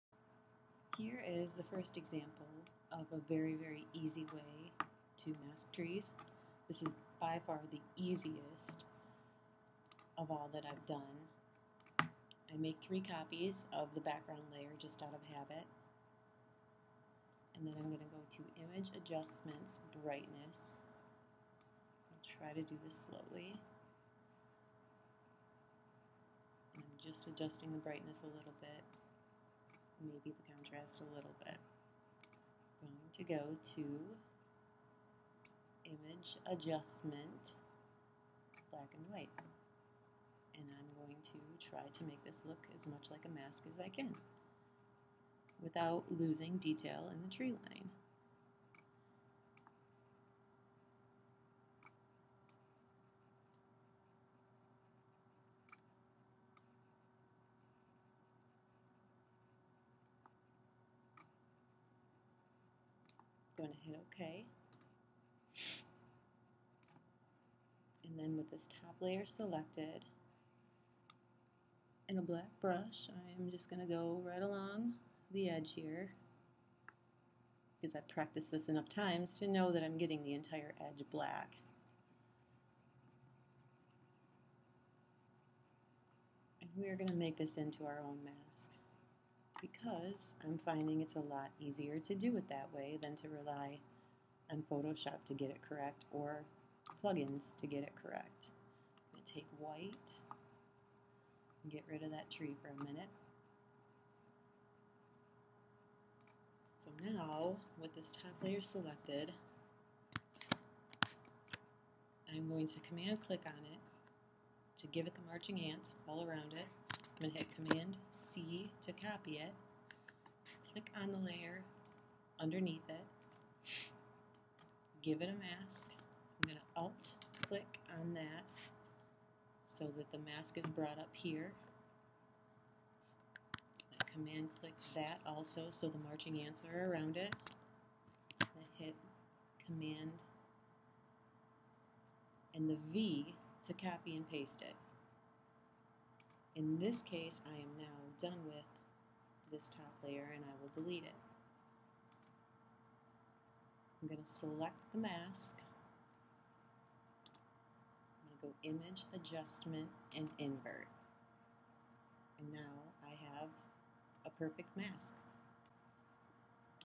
Tree masking for tracked night sky images in Photoshop
Using a star tracker blurs your foreground. You have to blend the static foreground with the tracked sky in photoshop. Here is the easiest way to mask the trees when you have a lot of trees in your composition.